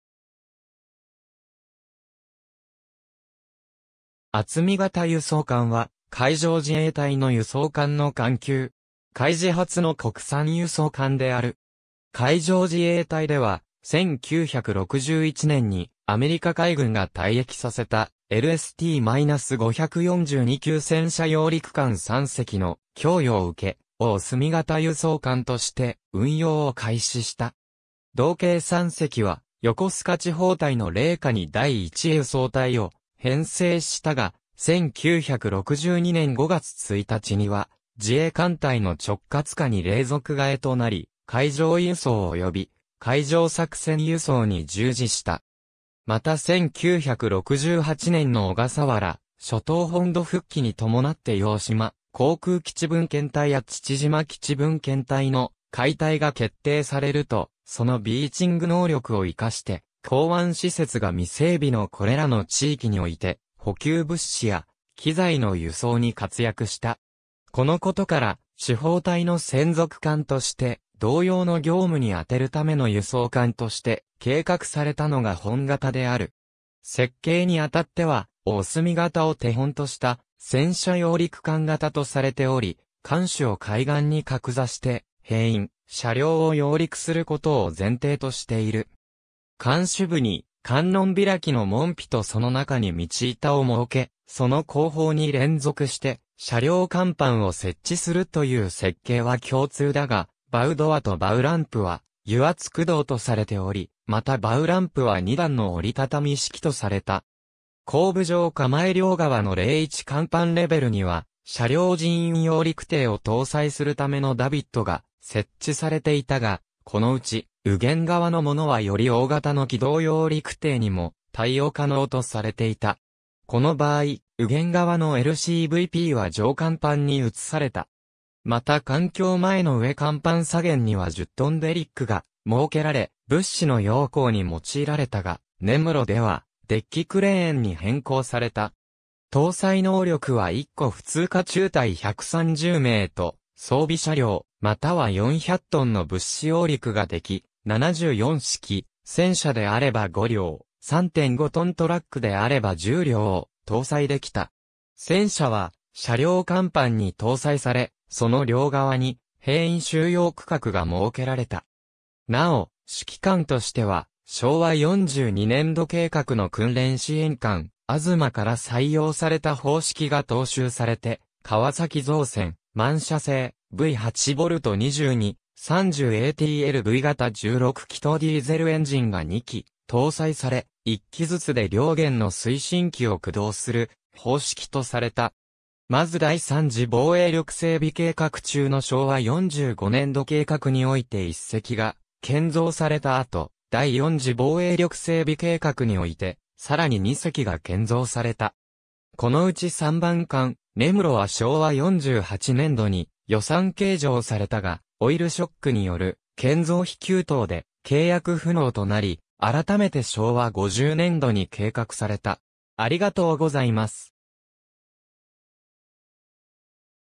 あつみ型輸送艦
あつみ型輸送艦（あつみがたゆそうかん、英語: Atsumi-class landing ship tank）は海上自衛隊の輸送艦の艦級。
海自初の国産輸送艦である。
海上自衛隊では、1961年にアメリカ海軍が退役させたLST-542級戦車揚陸艦（LST-1級最後期型）3隻の供与を受け、おおすみ型輸送艦として運用を開始した。
同型3隻は横須賀地方隊の隷下に第1輸送隊を編成したが、1962年5月1日には自衛艦隊の直轄下に隷属替えとなり、海上輸送及び海上作戦輸送に従事した。
また1968年の小笠原諸島本土復帰に伴って硫黄島航空基地分遣隊や父島基地分遣隊の開隊が決定されると、そのビーチング能力を生かして、港湾施設が未整備のこれらの地域において、補給物資や器材の輸送に活躍した。
このことから、地方隊の専属艦として同様の業務にあてるための輸送艦として計画されたのが本型である。
設計にあたっては、おおすみ型を手本とした戦車揚陸艦（LST）型とされており、艦首を海岸に擱座して兵員・車両を揚陸することを前提としている。
艦首部に観音開きの門扉（バウ・ドア）とその中に道板（バウ・ランプ）を設け、その後方に連続して車両甲板を設置するという設計は共通だが、バウ・ドアとバウ・ランプは油圧駆動とされており、またバウ・ランプは二段の折りたたみ式とされた。
後部上構両側の01甲板レベルには車両人員揚陸艇（LCVP）を搭載するためのダビットが設置されていたが、このうち右舷側のものはより大型の機動揚陸艇（LCM）にも対応可能とされていた。
この場合、右舷側のLCVPは上甲板に移された。
また艦橋前の上甲板左舷には10トンデリックが設けられ、物資の揚降に用いられたが、「ねむろ」ではデッキクレーンに変更された。
搭載能力は1個普通科中隊130名と装備車両、または400トンの物資揚陸ができ、74式戦車であれば5両、3.5トン・トラックであれば10両を搭載できた。
戦車は車両甲板に搭載され、その両側に兵員収容区画が設けられた。
なお主機関としては、昭和42年度計画の訓練支援艦「あづま」（42ATS）から採用された方式が踏襲されて、川崎造船・MAN社製V8V22/30ATL V型16気筒ディーゼルエンジンが2基搭載され、1基ずつで両舷の推進器を駆動する方式とされた。
まず第3次防衛力整備計画中の昭和45年度計画において1隻が建造された後、第4次防衛力整備計画においてさらに2隻が建造された。
このうち3番艦「ねむろ」は昭和48年度に予算計上されたが、オイルショックによる建造費急騰で契約不能となり、改めて昭和50年度に計画された。